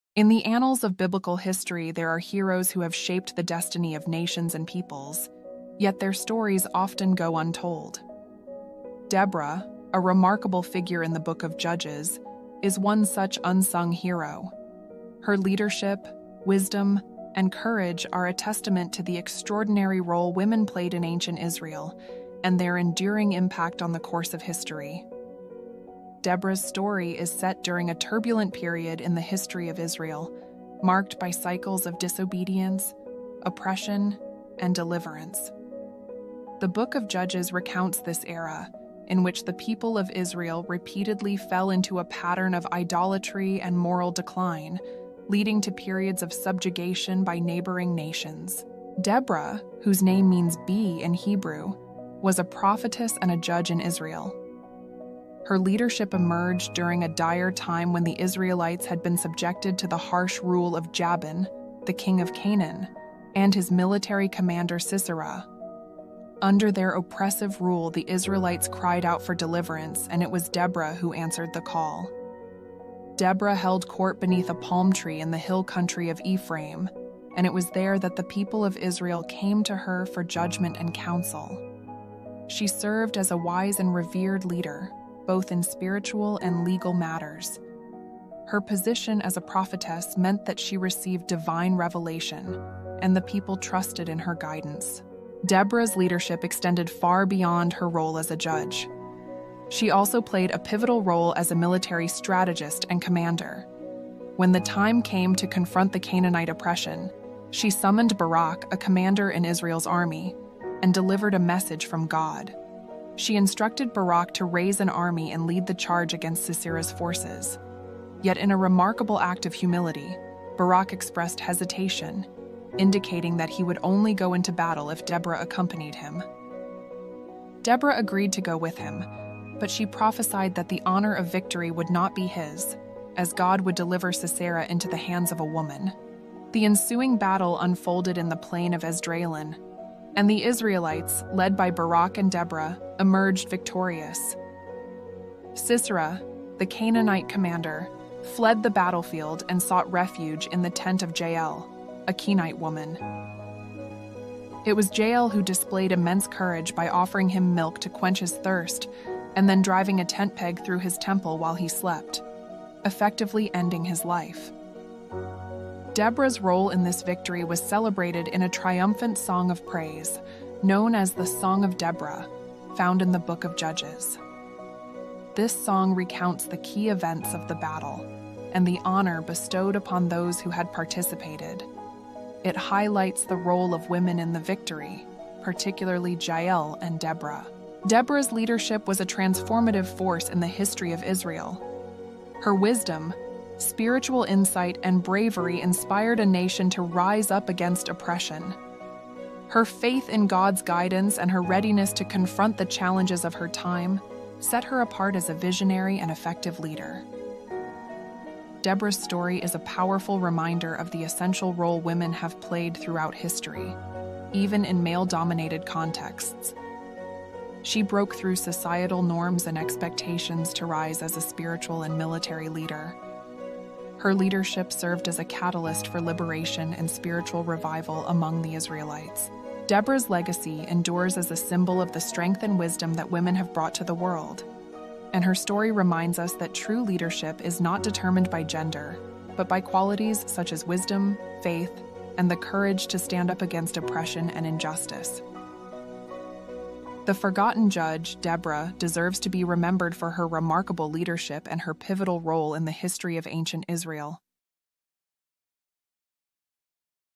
The Forgotten Judge | The Leadership of DEBORAH | Bible Stories
Discover the compelling story of Judge Deborah, a remarkable but often overlooked figure in the Bible. Her unwavering leadership and wisdom set her apart as one of the most prominent women in biblical history. Journey through the accounts of her time as a judge and prophetess, her pivotal role in guiding the Israelites to victory, and her enduring legacy. Learn about the lessons of courage, faith, and leadership that Deborah's story imparts.